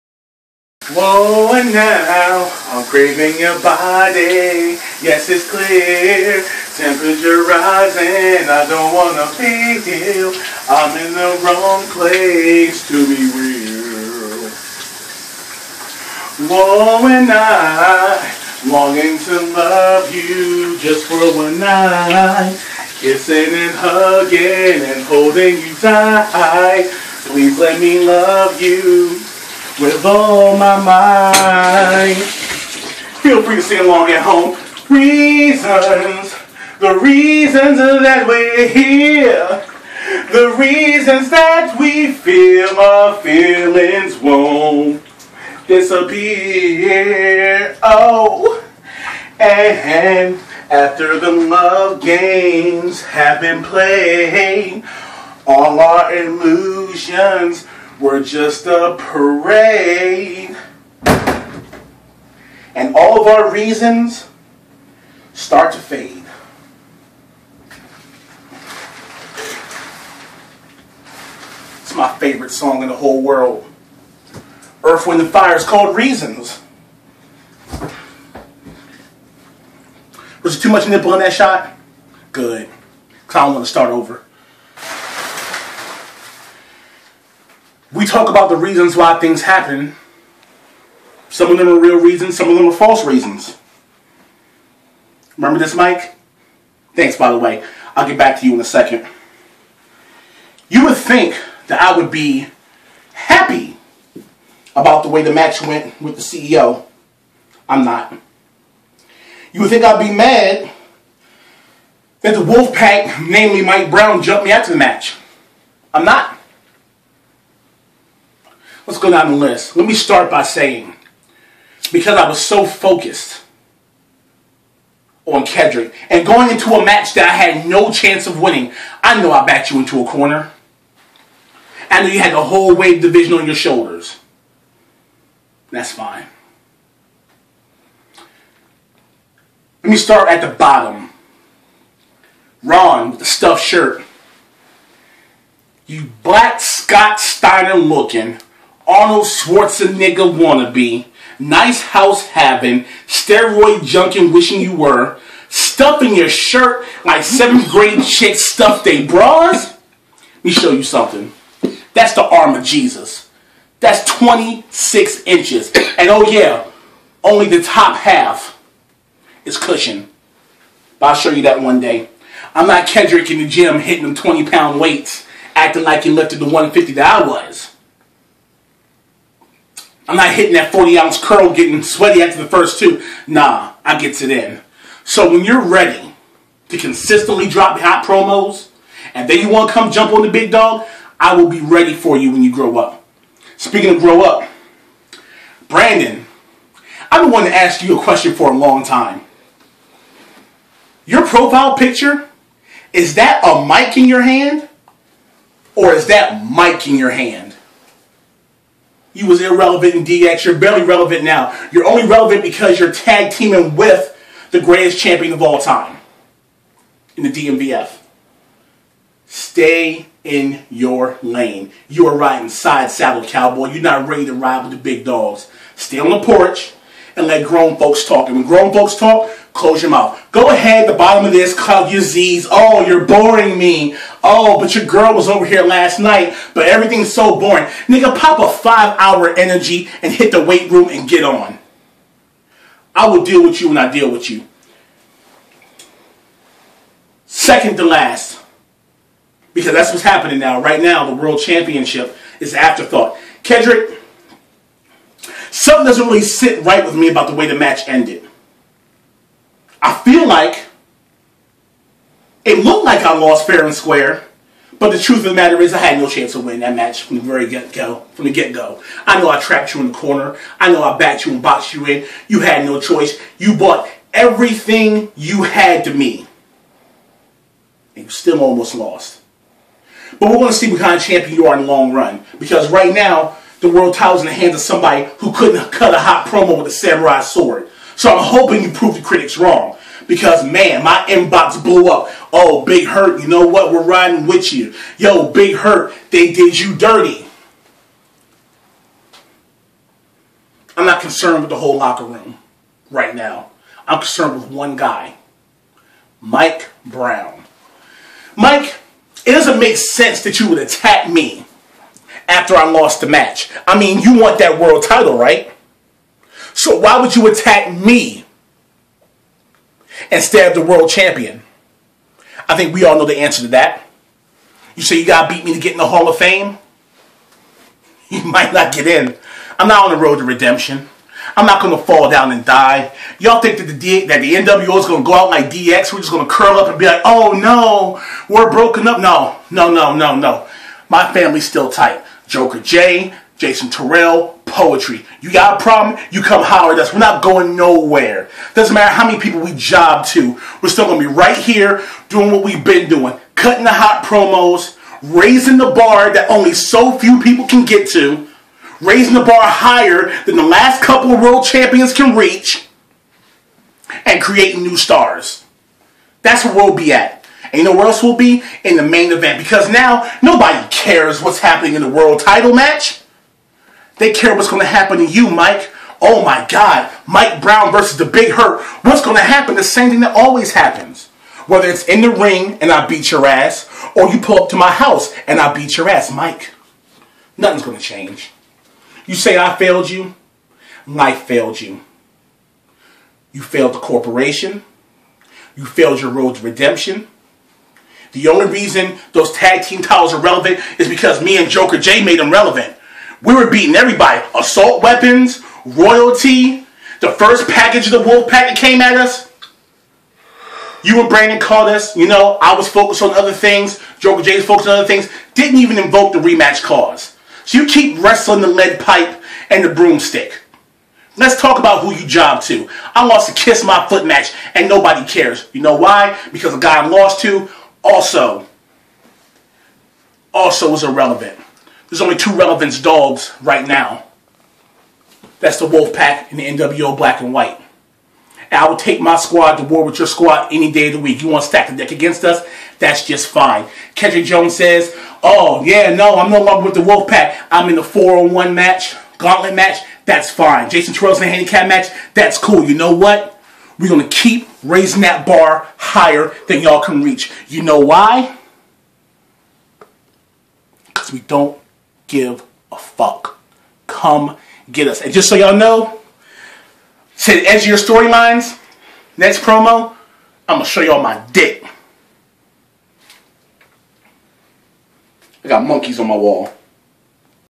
The Big Hurt - The Reasons Promo
The Big Hurt discusses the REASONS for his downward spiral.